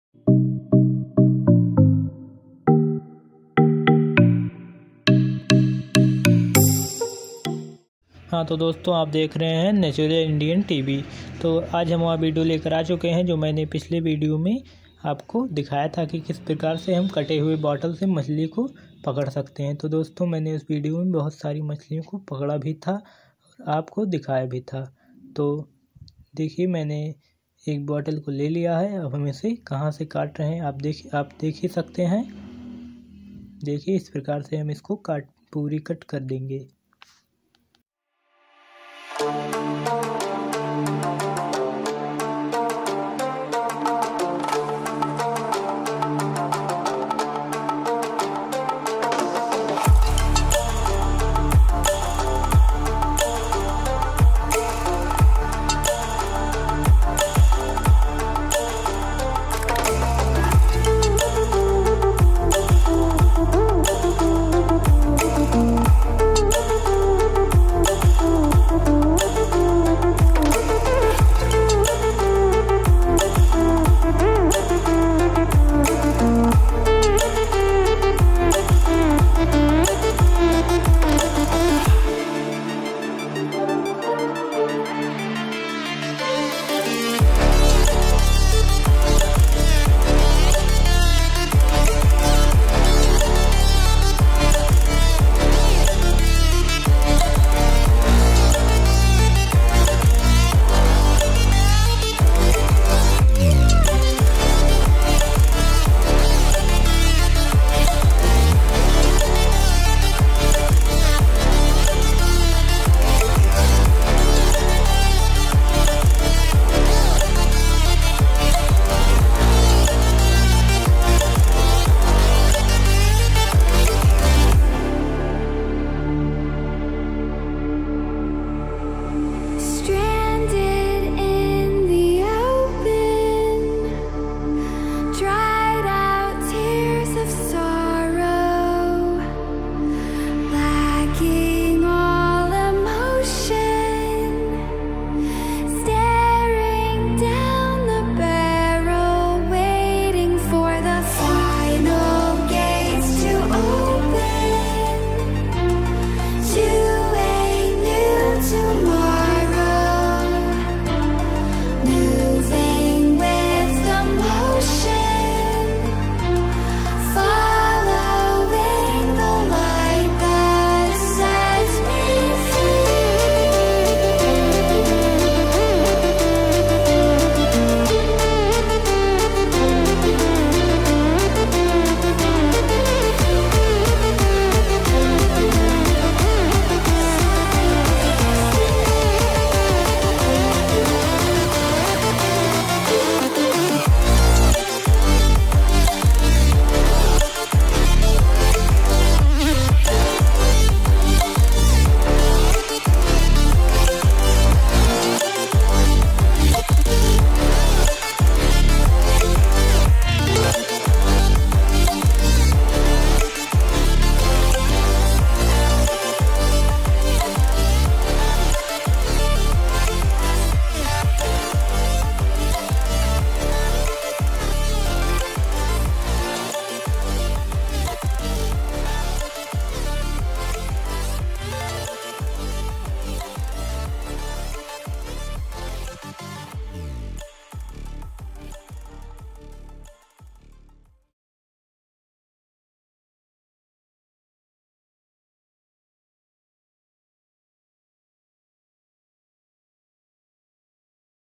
मछली पकड़ने का देसी जुगाड़/मछली पकड़ने का नया तरीका/Amazing fish catch with a plastic bottle (2021)
आज हम इस वीडियो के माध्यम से आपको बताएंगे कि मछली को कैसे पकड़े